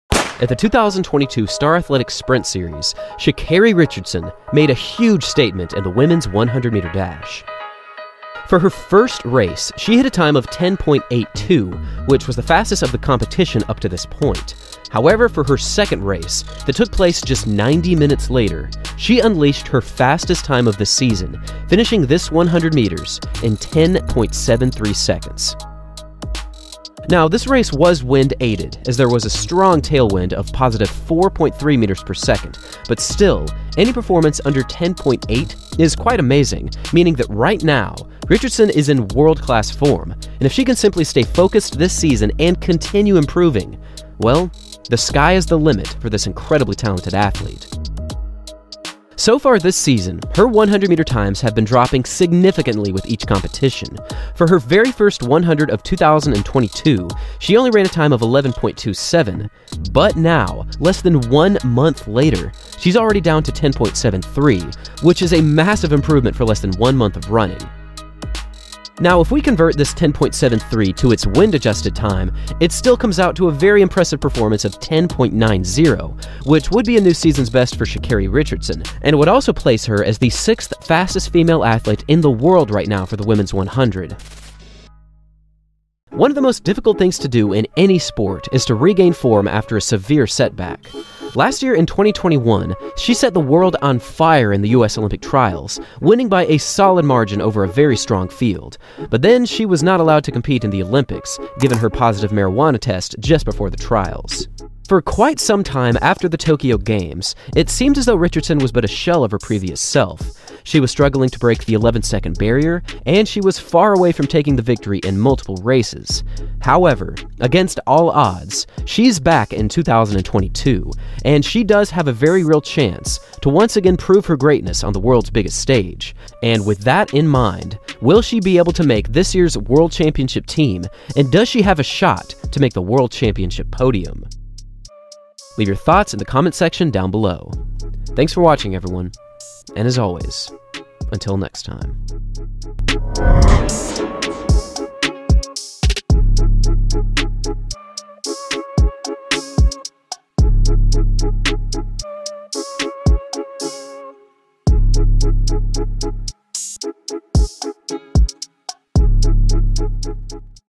Sha'Carri Richardson Unleashes Incredible 100 Meter Time || 2022 Star Athletics Sprint Series #1
After a wild 2021 track and field campagin, Richardson has returned with a vengeance, and her 2022 season is looking amazing.